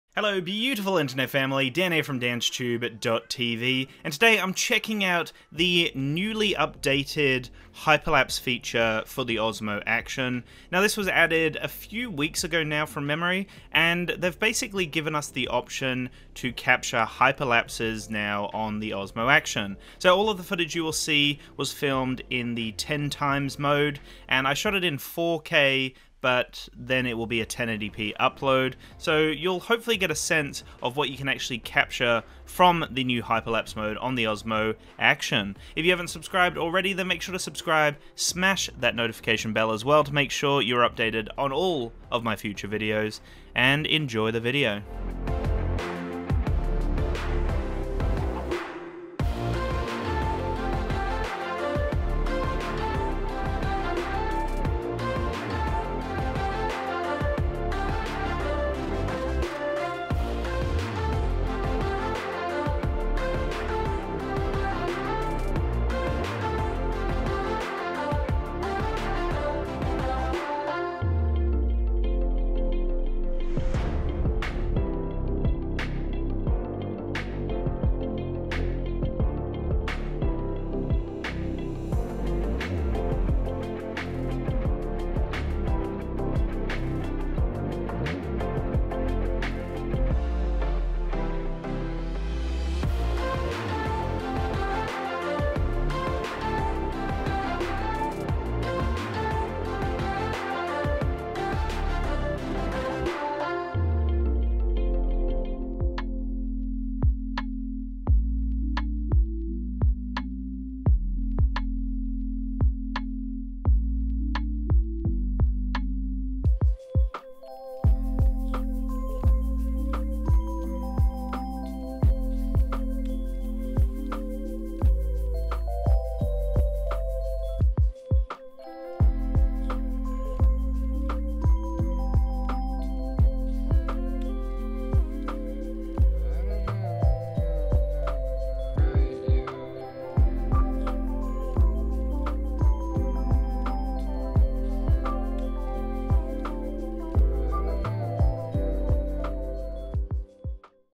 NEW Osmo Action Hyperlapse Feature | DansTube.TV
The Osmo Action was recently updated to offer a hyperlapse feature. This update was hot on the heels of the GoPro Hero 8 release, sneaky sneaky DJI ;) BUY Osmo Action

The update also included:

*Added Hyperlapse
*Added time settings for turning the touchscreen off automatically while recording
*Added Thia and Arabic languages for Osmo Action
*Reduced the live view latency when video resolution was set to 2.7K and 1080p with 24/25/30 fps.

Primary camera
On-camera microphone
Studio microphone